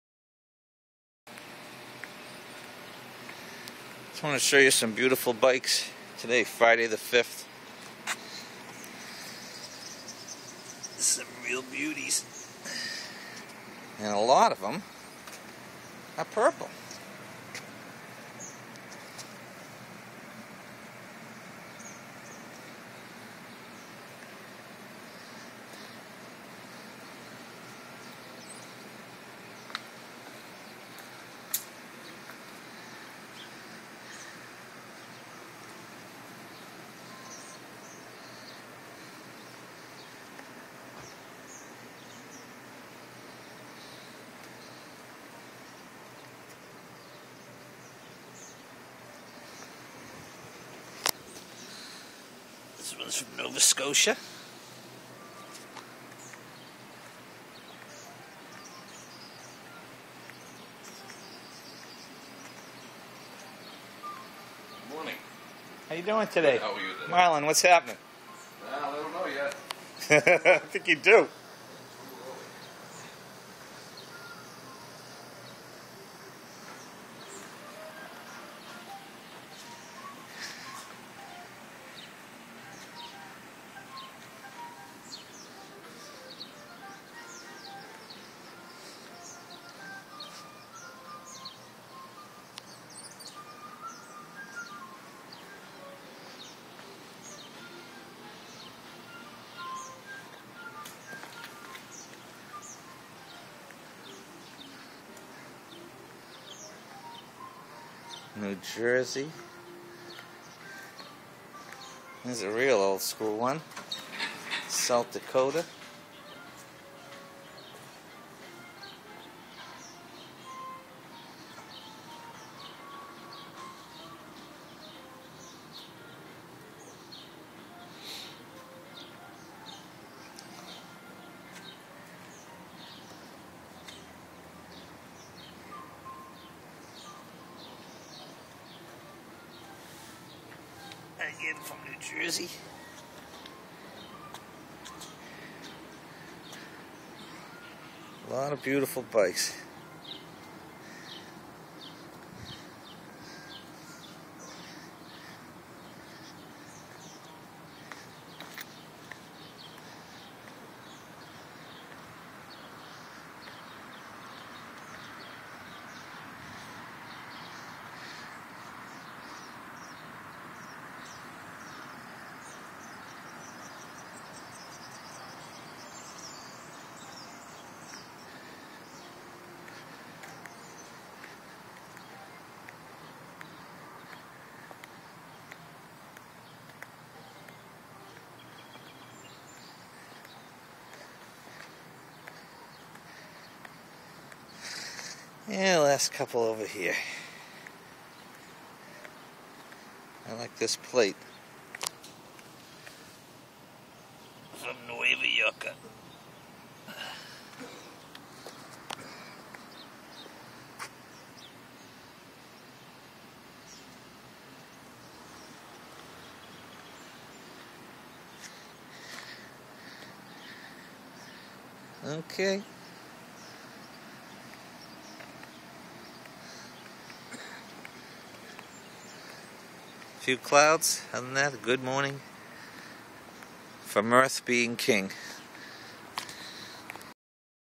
Jester Bikes
Here are a few examples of ROJ motorcycles AKA Purple Stallions. Enjoy